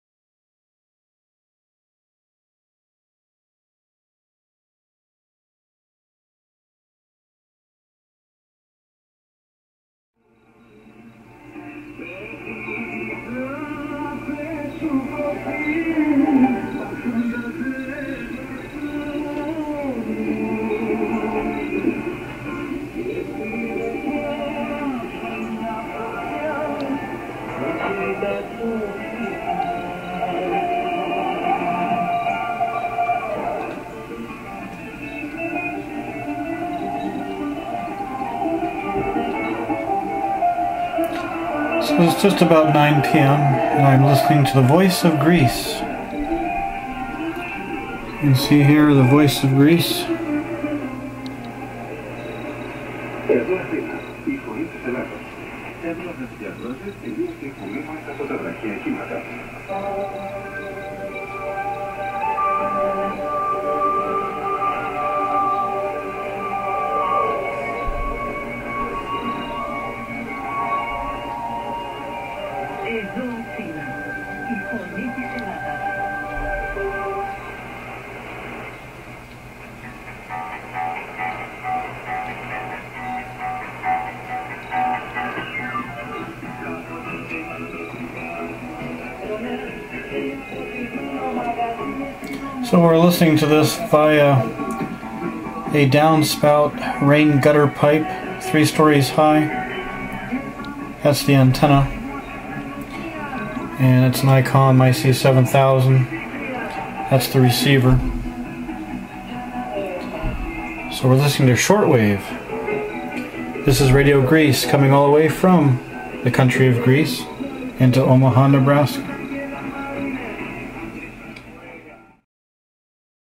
Voice of Greece on Shortwave Radio 9420 kHz 16-May-2012 (SWL in Omaha, NE)
Reception of "The Voice of Greece" transmitting on 9420 kHz in the 31-meter band, on shortwave radio.  The receiver is an Icom IC-7000 located in Omaha, Nebraska, using a rain gutter and down-spout, three stories high, as the antenna.  The transmitter is using 170,000 Watts of power.(170 kW RF), and is located in Greece.  The language is Greek.

Cheers,
Tomas / NW7US